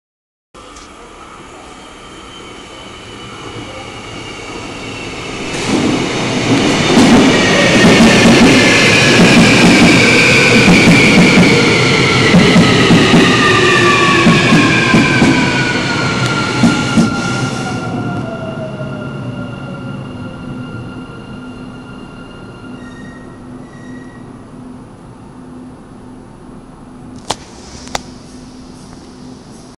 부산교통공사 2호선 동원역 진입영상
이 영상도 마찬가지로 2012년 여름에 촬영했습니다.
게다가 차량소자가 IGBT군요;